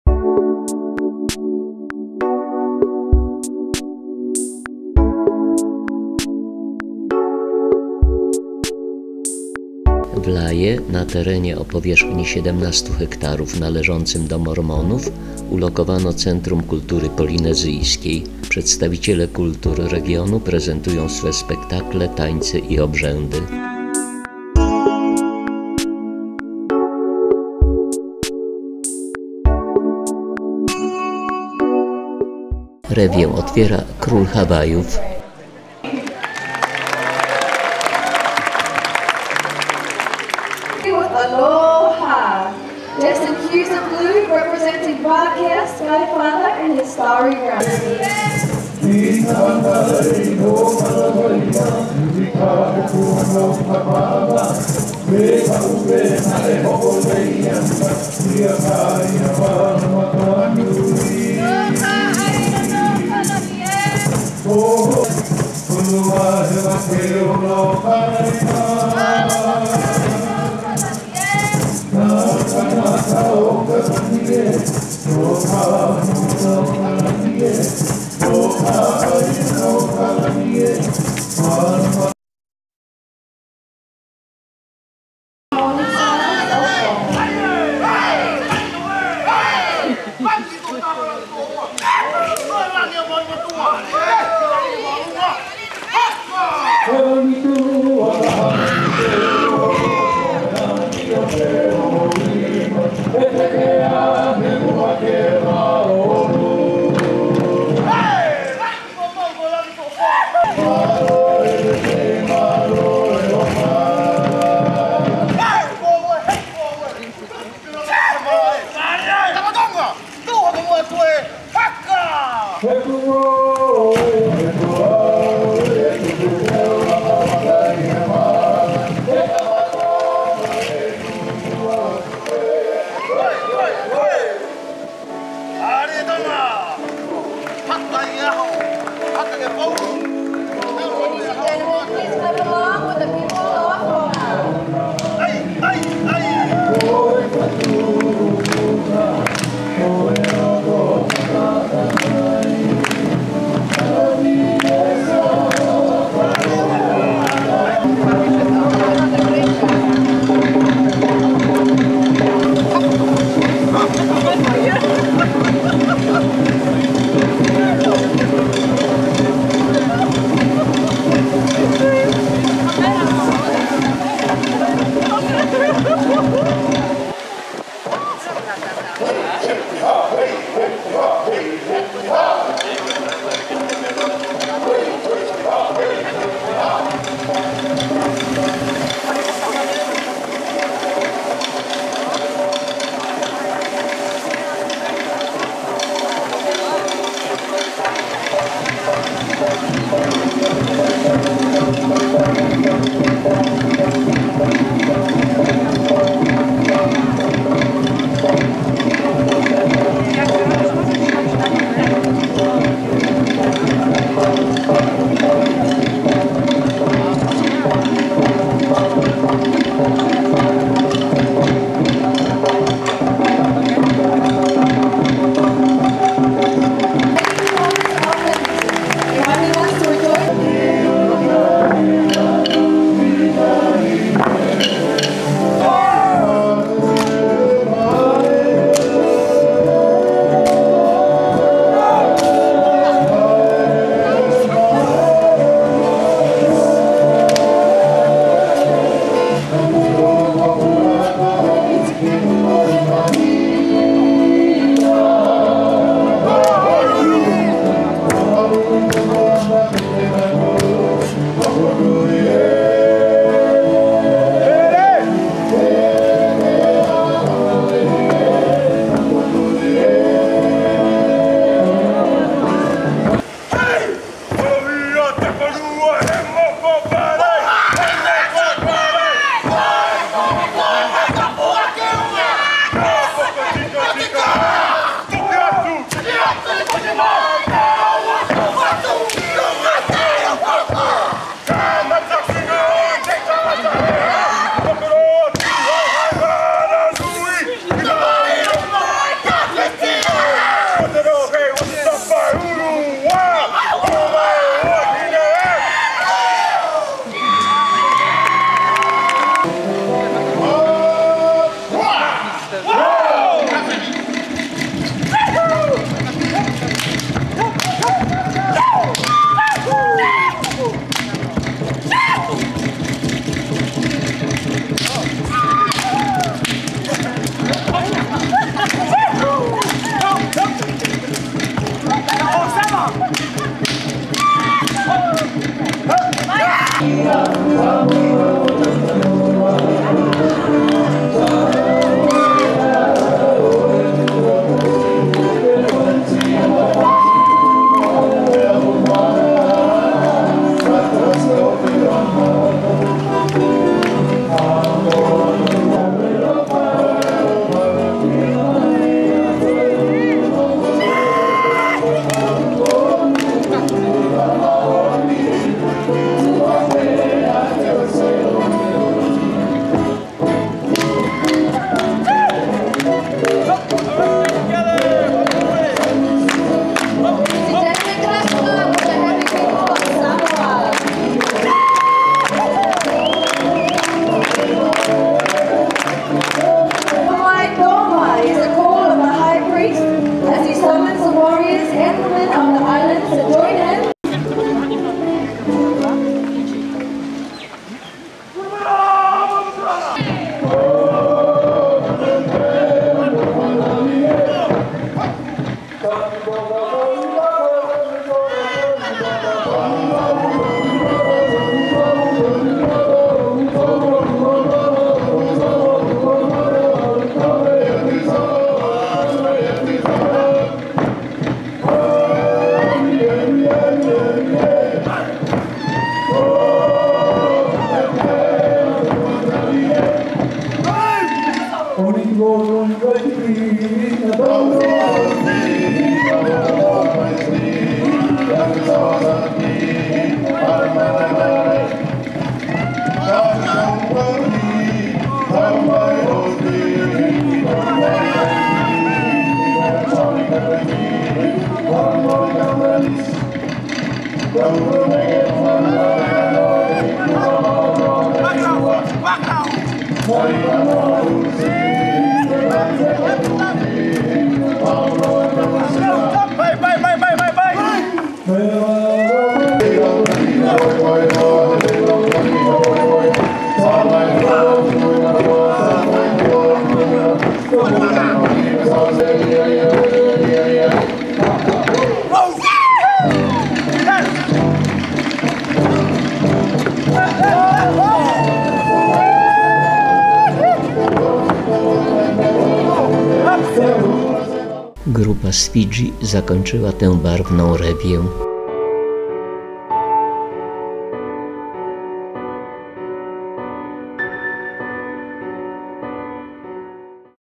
Hawaje - Rewia grup tanecznych z wysp Polinezji
W Laie niedaleko Honolulu, na terenie 17 hektarów należącym do Mormonów, ulokowano Centrum Kultury Polinezyjskiej. Przedstawiciele kultur regionu prezentują tu swe spektakle, tańce i obrzędy.
Na płynących wodą tratwach wystąpią kolejno przedstawiciele Hawajów, Tonga, Tahiti, Aotearoa (Nowa Zelandia), Samoa i Fiji.